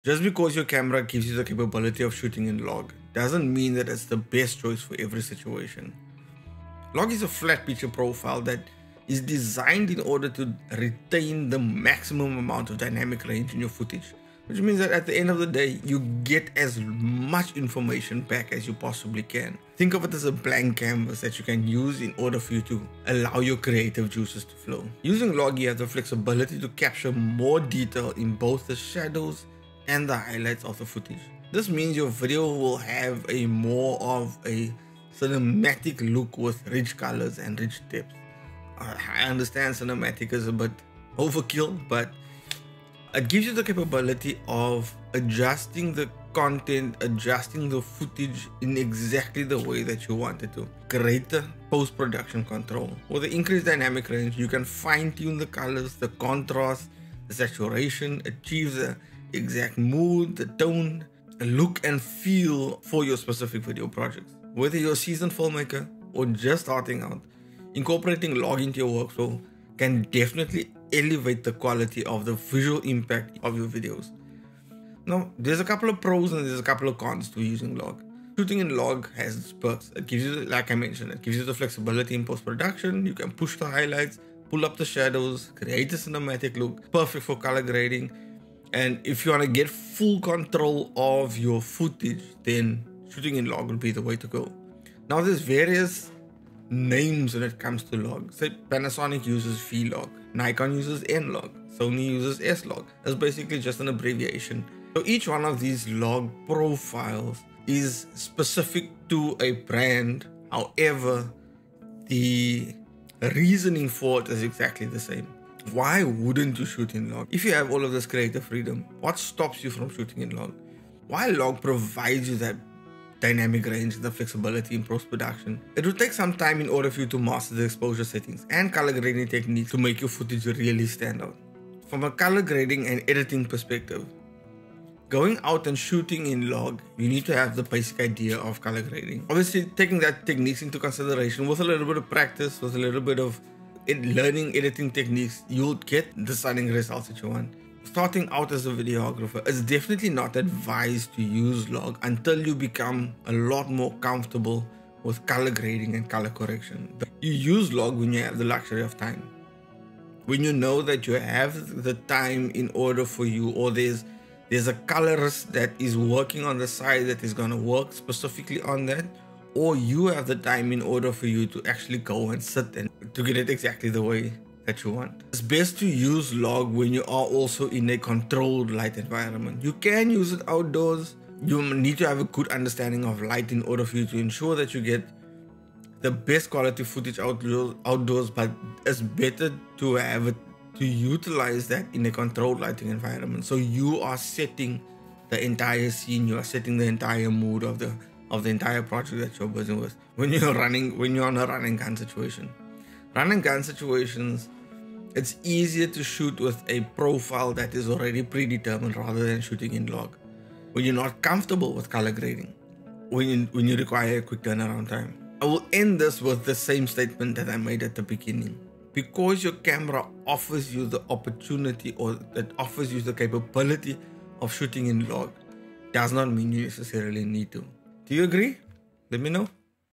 Not always a good thing
The log capability on a camera give you a lot of control, but when is it a good time to use it?

Must have gear, best options
My Current Gear List (Best Deals)

Checkout my Lightroom Preset Packs on ETSY